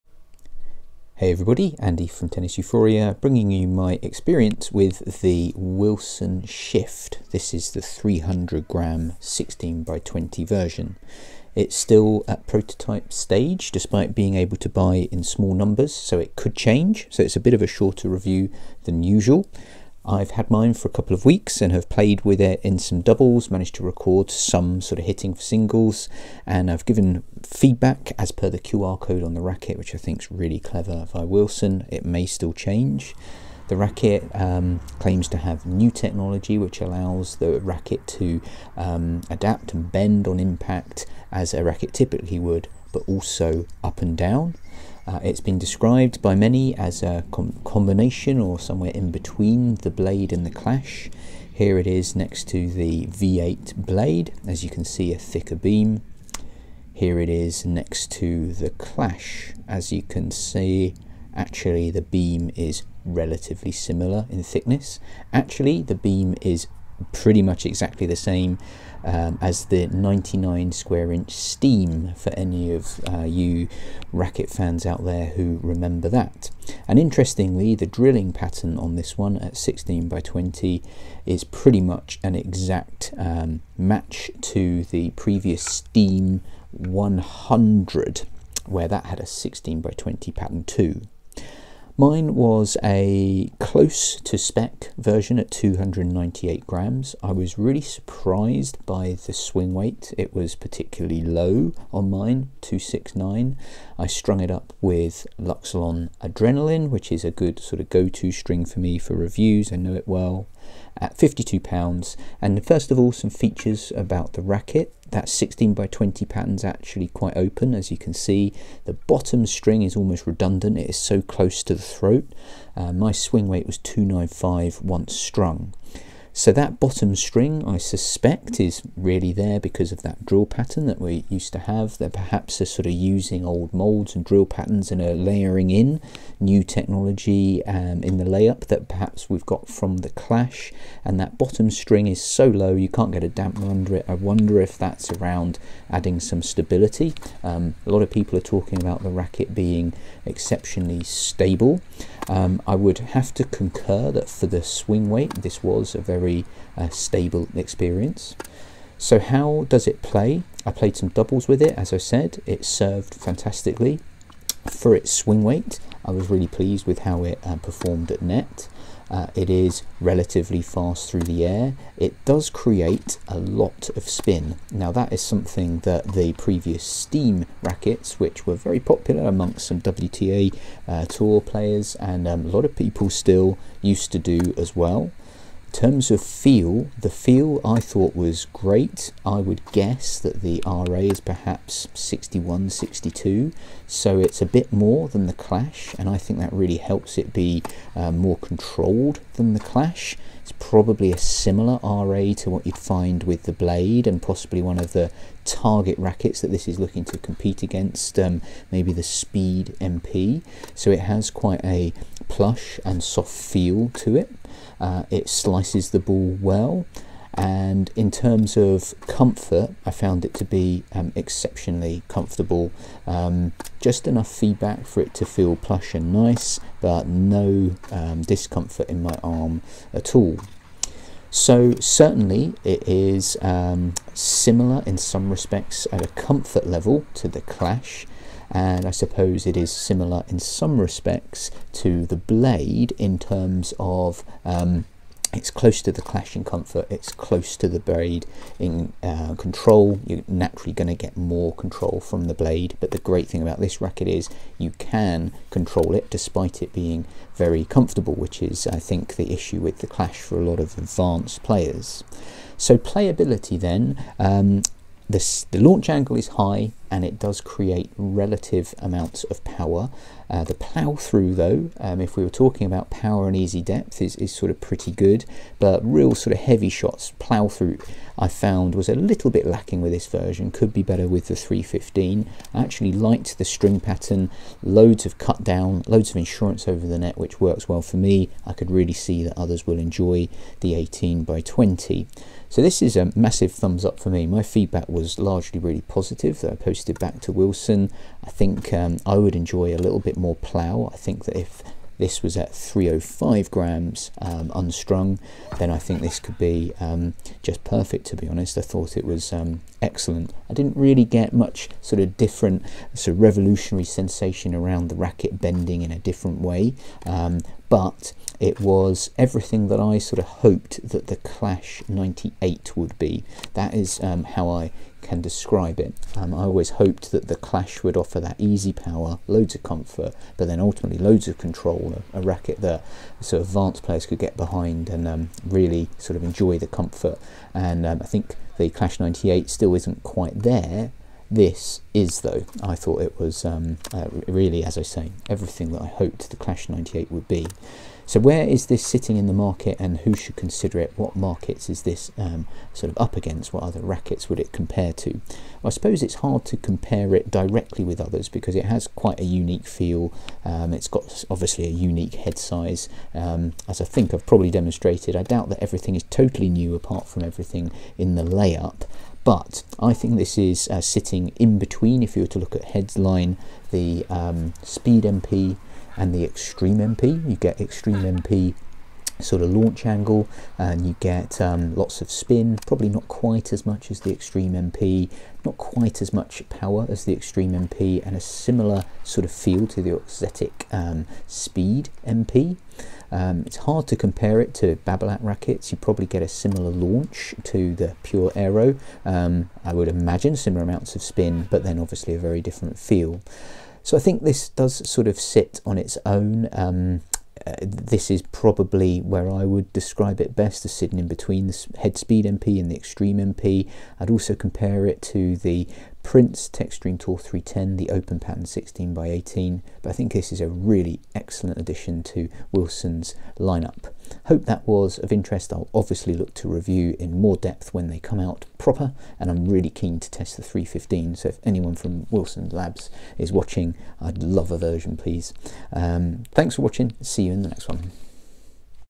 Wilson Shift 300g | 16x20 tennis racket / racquet review
A review of the Wilson Shift tennis racket which at this point remains somewhat of a pilot / prototype. In the video I discuss its attributes, technology, feature and look to place it in the market. I also hit with it to review its performance and feedback on its playing style.